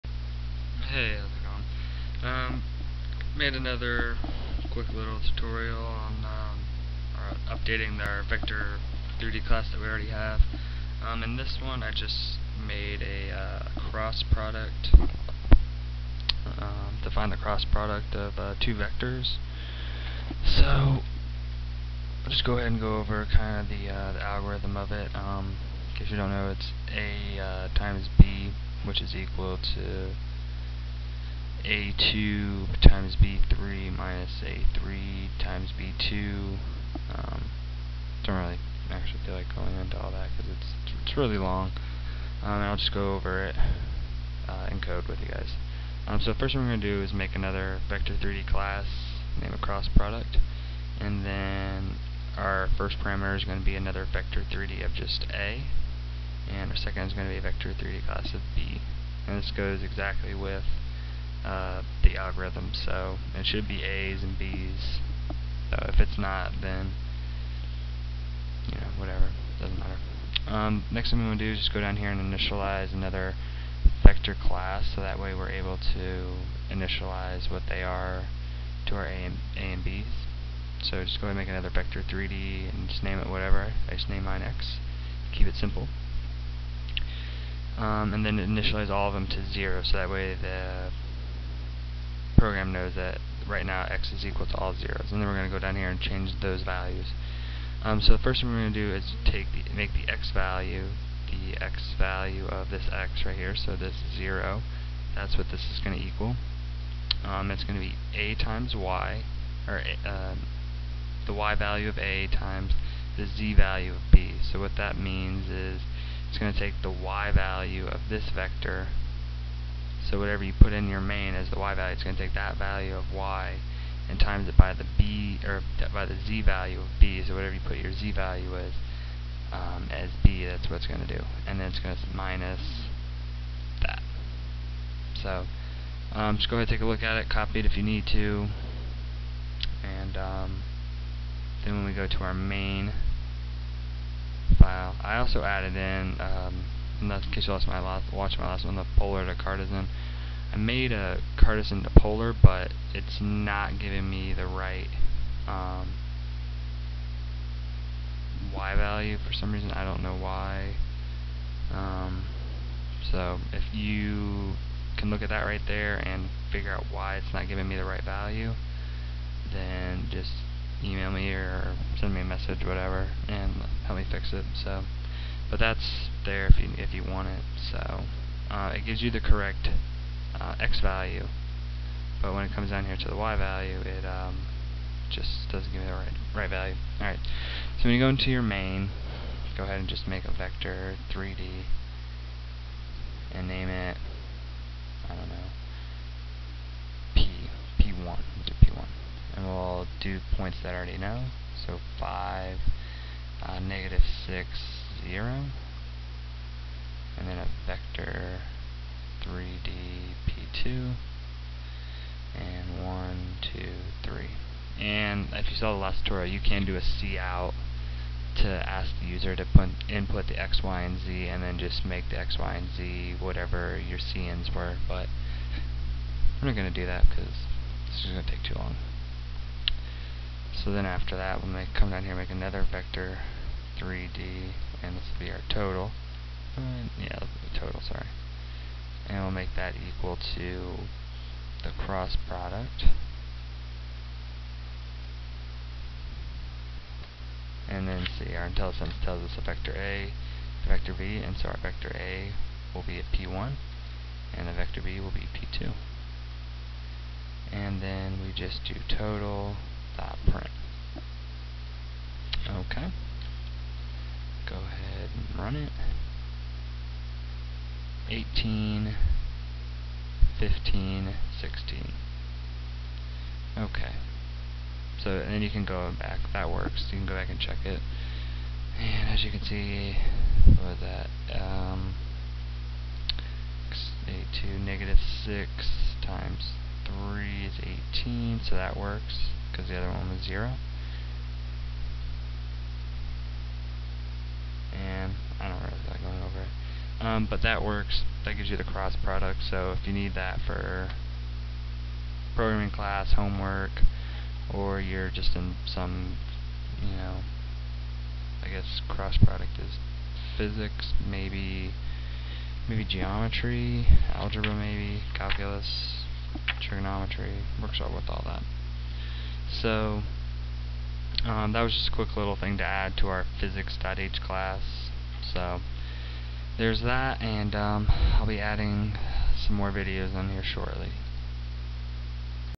Advanced C++ Tutorials 2 : Vectors & Physics : Cross Product
A helpful way to learn c++, and to understand how to program physics in programming. Part 2 goes over making a function for cross product.